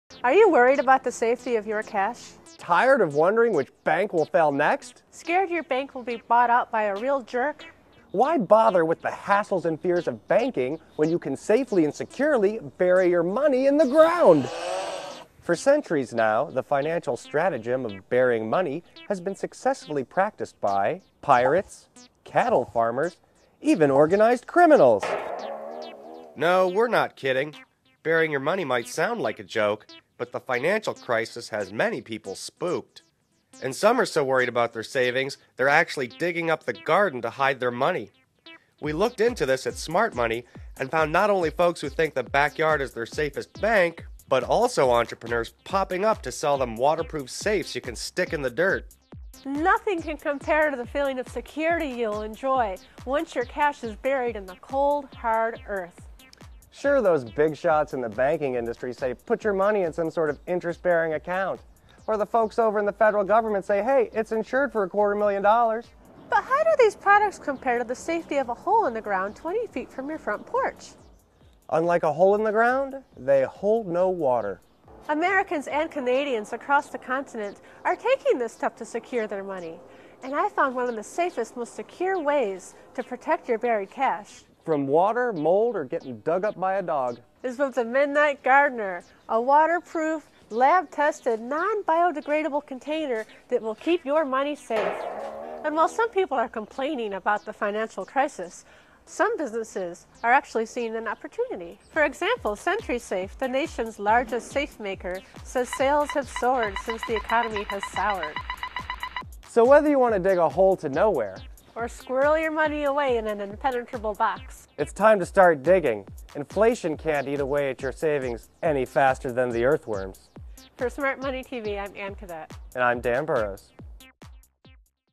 Burying Your Cash
Anne Kadet and Dan Burrows take an in-depth look at the burgeoning industry of underground valuables-storage.